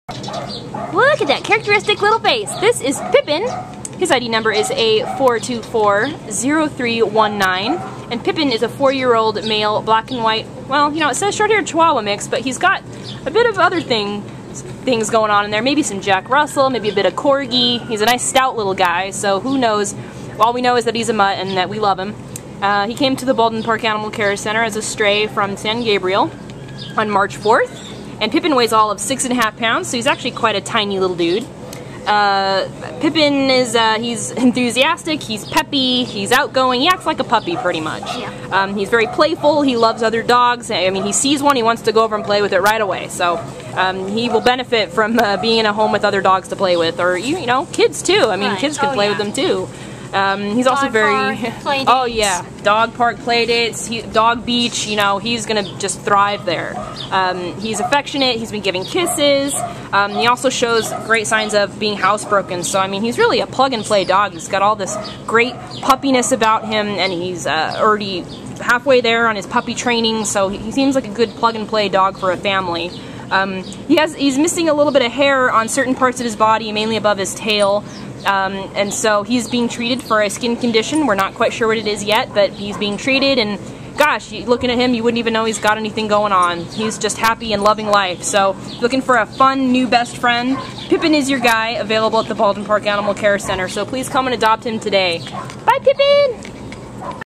A4240319 Pippin
Mission:
A3337216 Pippin is a tiny, ready-to-go 4-year-old white-with-black Chihuahua and Jack Russell Terrier mix who came to the Baldwin Park Animal Care Center as a stray from San Gabriel on March 4th. Weighing a diminutive 6½ lbs, Pippin is the quintessential 4-years-young mutt—enthusiastic, outgoing, affectionate (he adores giving and getting kisses), and cute as a button. He was happy to walk with his handler or spend time in her arms or lap, and he appears to like the company of other dogs, whom he sought out, seemingly for friendship and playtime. Pippin’s currently being treated for a bit of alopecia, a skin condition that causes loss of hair, and is coming along nicely. He walks great on leash (as if he’s had lots of practice), shows signs of being housebroken, and is extremely cute and colorful (he has sort of a cow pattern, with unusual cross-overs of his two colors). Little Pippin is what we call a plug-and-play; as such, he will most likely fit readily and easily as an indoor pet and companion in the home of any individual or family with kids old enough to respect his mini-size. We think he’s ready to be a favorite family member, accompanying his new person or persons on walks and serving as a purse-buddy for outings to just about any place.